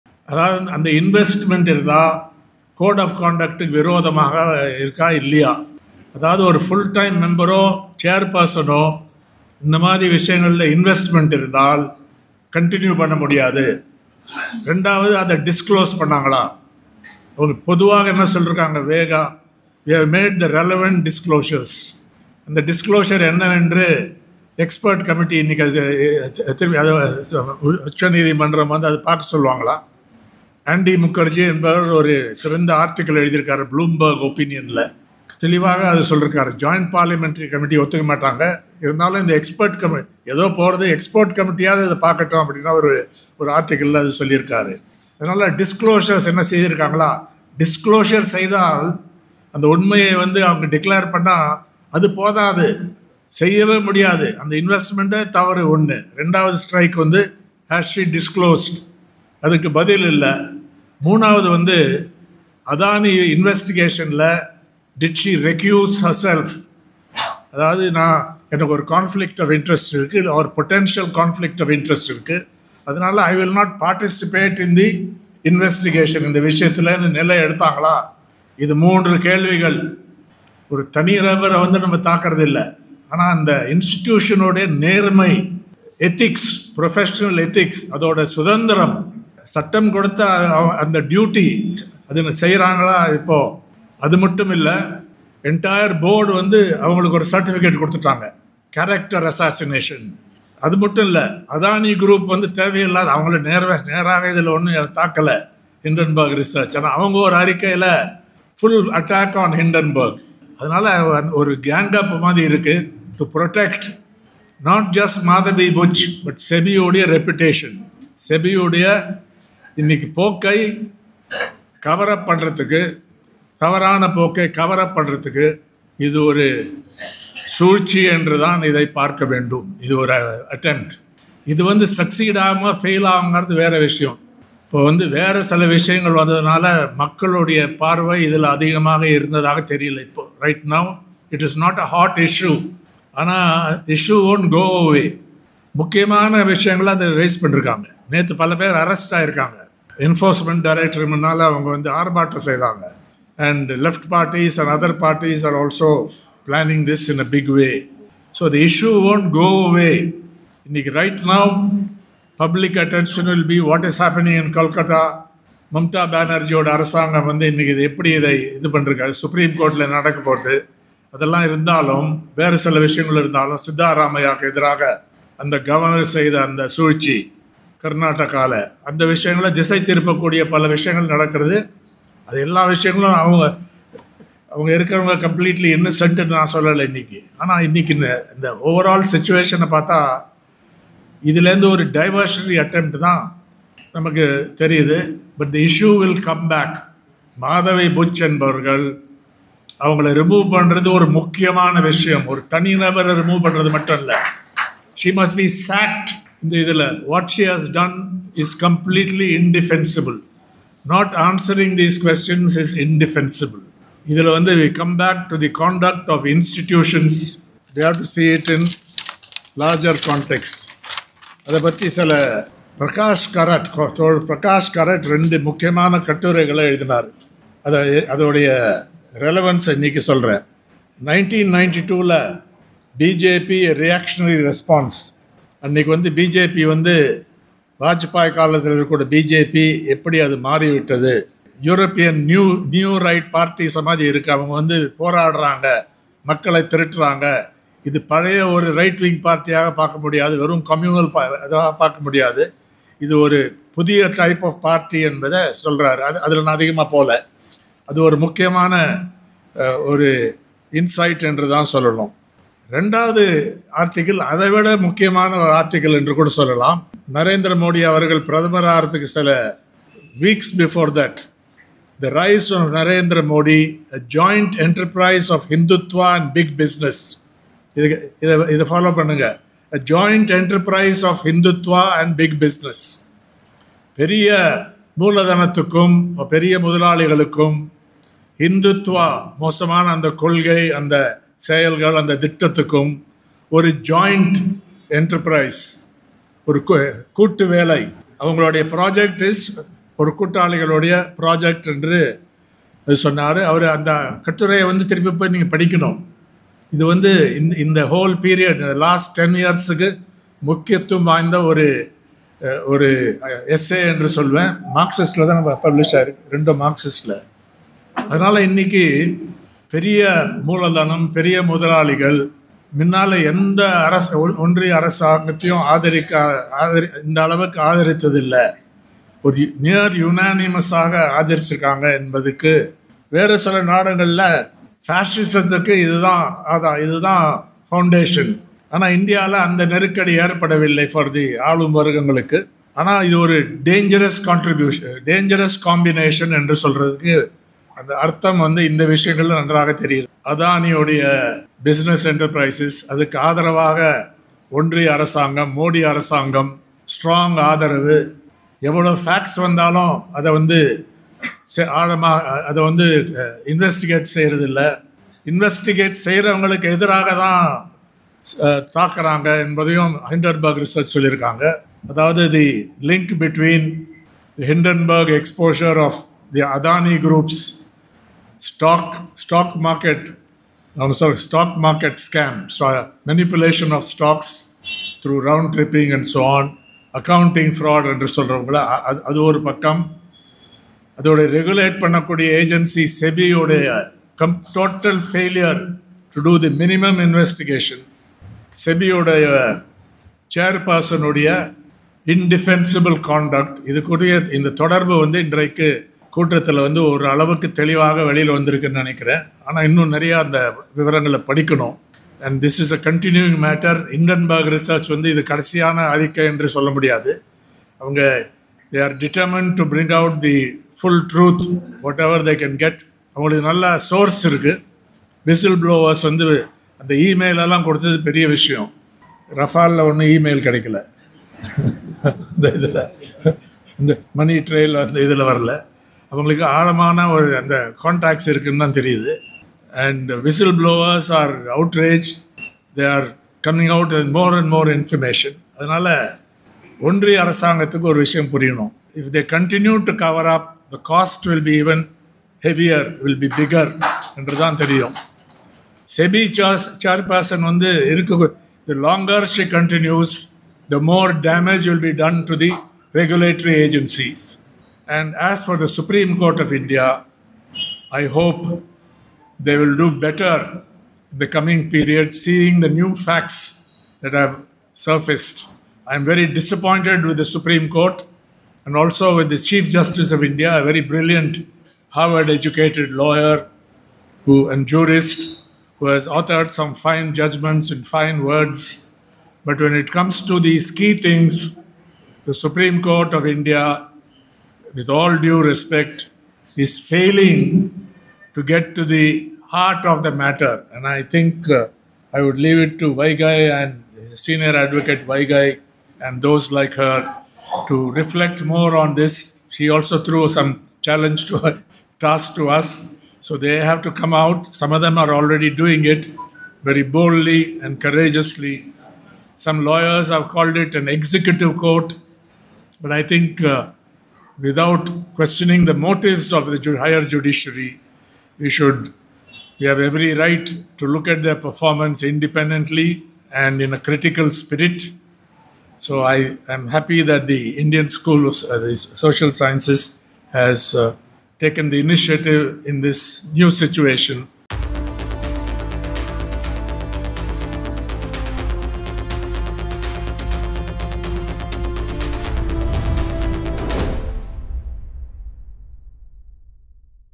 அதானி குழும முறைகேட்டில் உடந்தையாக இருந்த செபி தலைவரை பதவி நீக்கம் செய்ய வேண்டும்...
அதானி குழும முறைகேட்டில் உடந்தையாக இருந்த செபி தலைவரை பதவி நீக்கம் செய்ய வேண்டும் - என்.ராம், முத்த பத்திரிக்கையாளர்